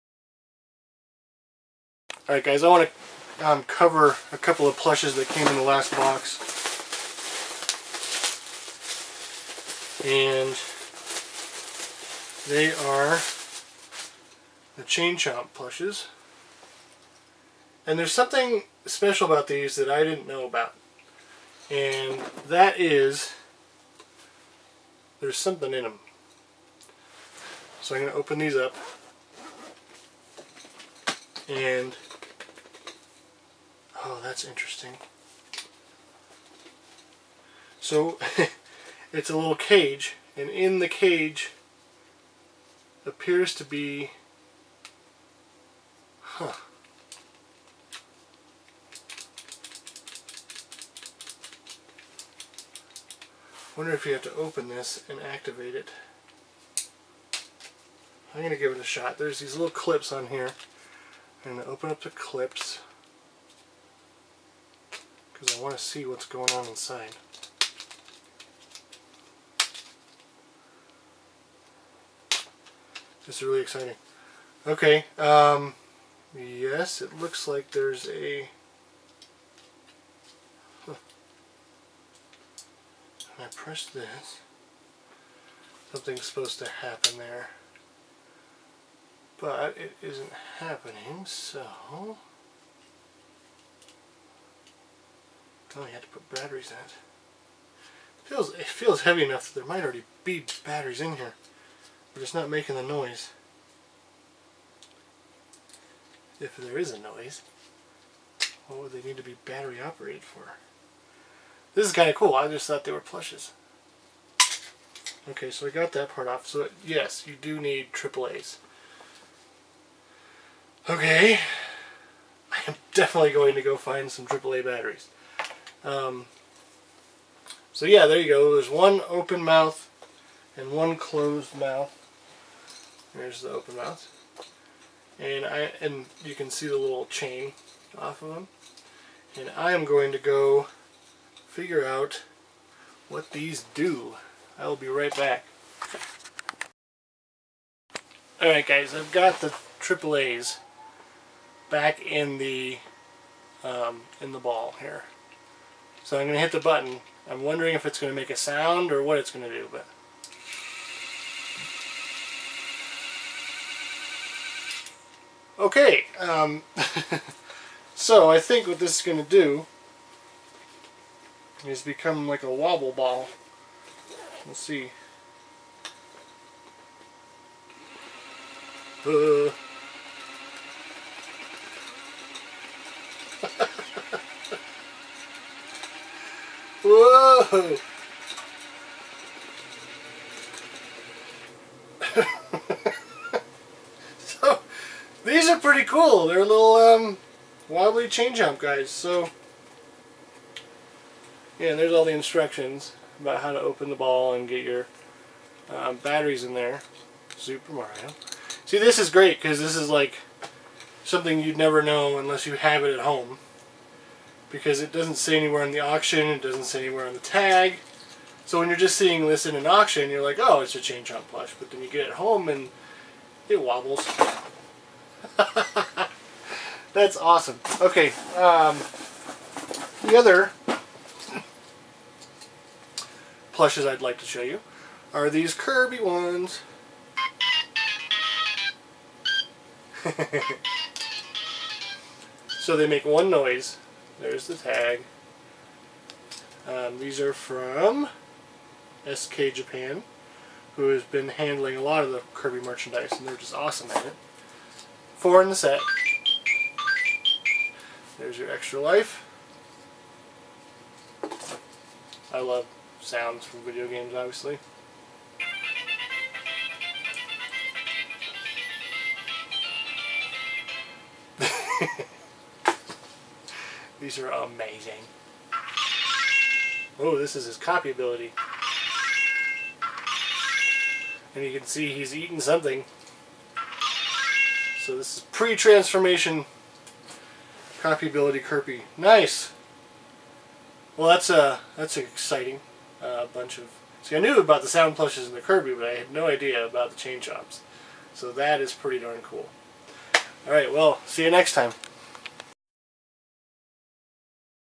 Chain Chomp plushes surprise me greatly, and some Kirby sound plushes!
These are some cool new Chain Chomp plushes - with a major surprise!